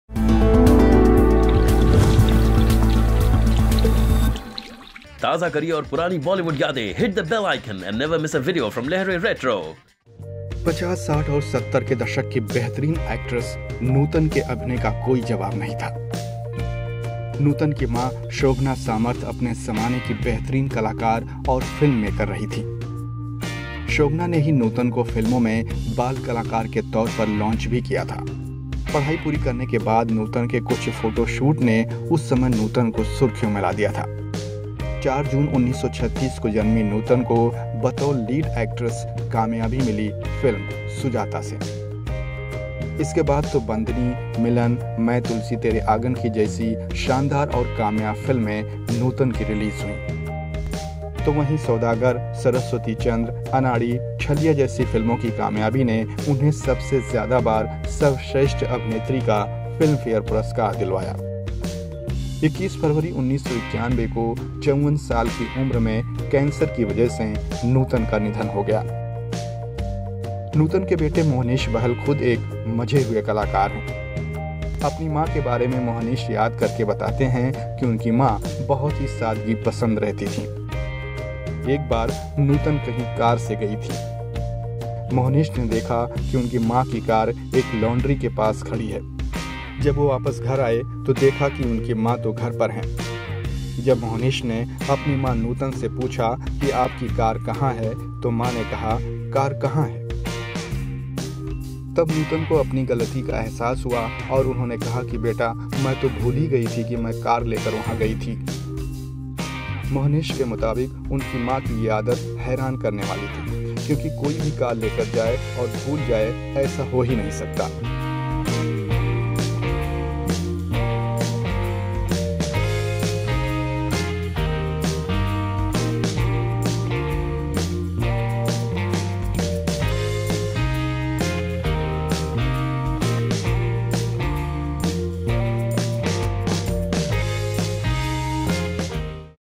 When Nutan SURPRISED Mohnish Bahl!
Yesteryear's actress late Nutan was one of the finest actresses of Hindi Cinema. But she was so humble. Check out the story how Nutan surprised her son Mohnish Bahl. About Lehren: